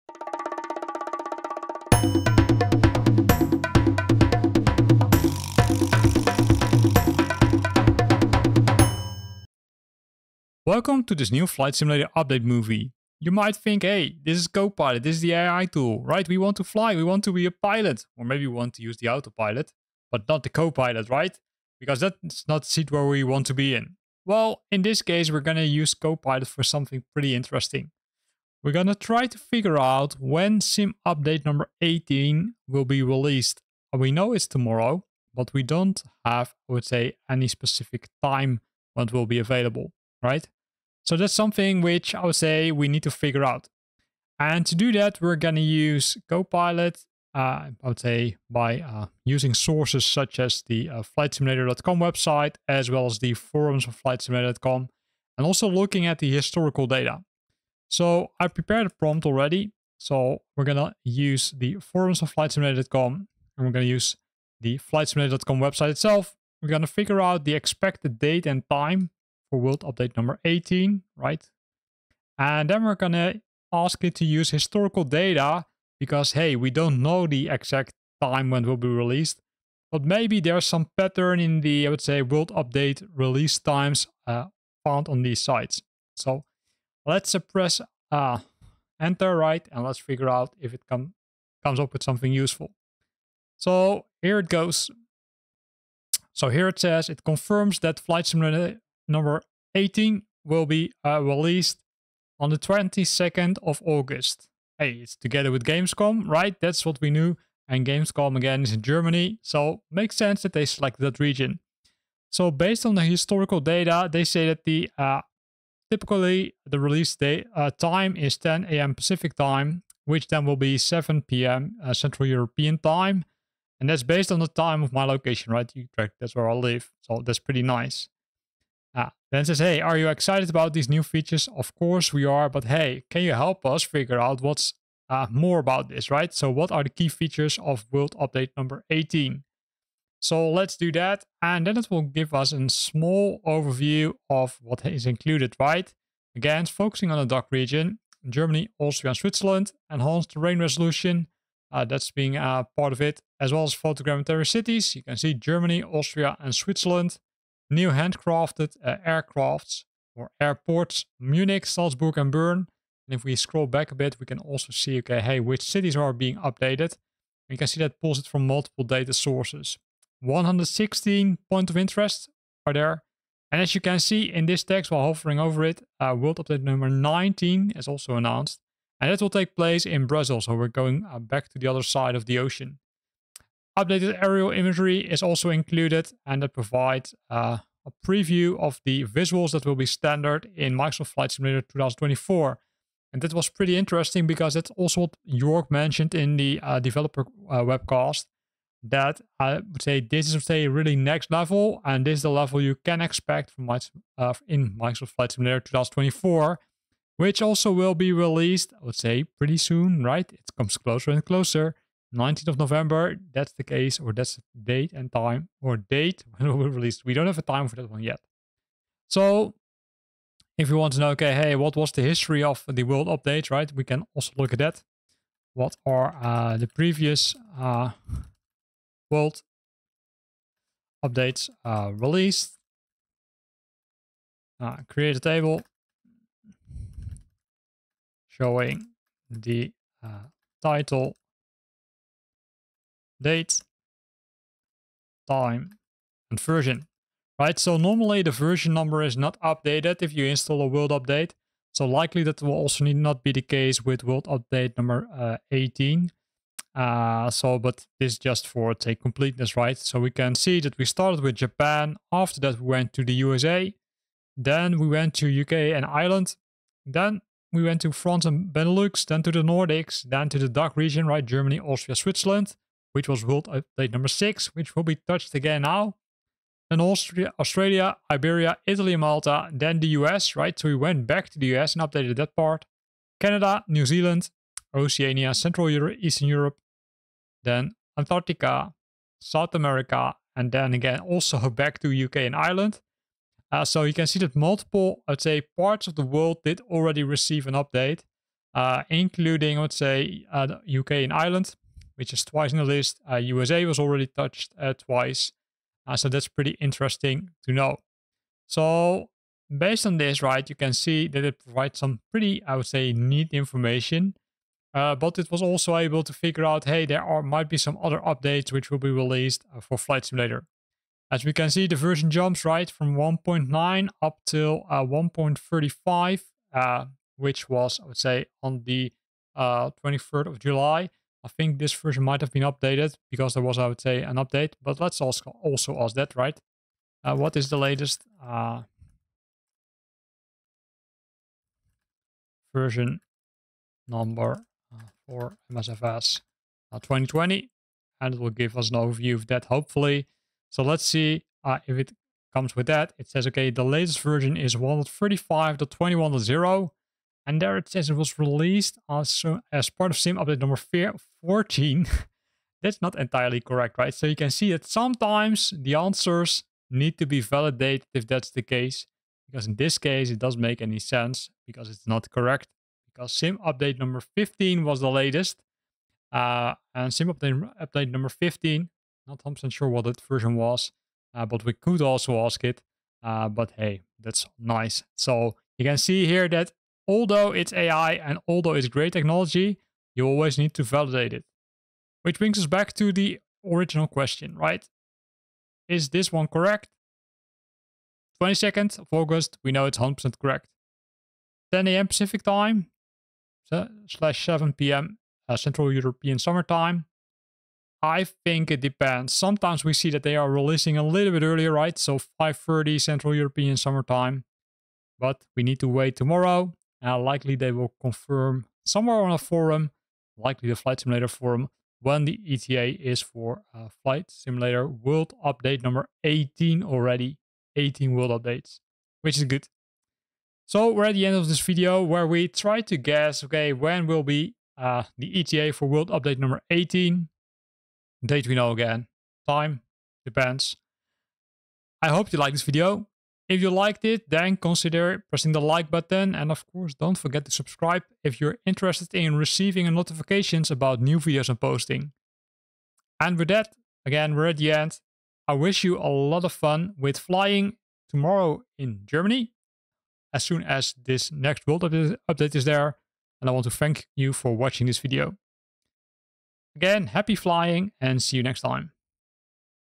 Microsoft Flight Simulator World Update 18: Release Date & Key Features Revealed!
Get ready, flight sim enthusiasts! In this video, we dive into the highly anticipated World Update 18 for Microsoft Flight Simulator, set to release on August 22, 2024, during Gamescom. We’ll cover the expected release time, key features, and enhancements coming to the DACH region (Germany, Austria, Switzerland). From stunning photogrammetry cities to new hand-crafted airports and updated aerial imagery, this update is packed with exciting content. Don’t miss out on the latest details and prepare for an immersive flying experience!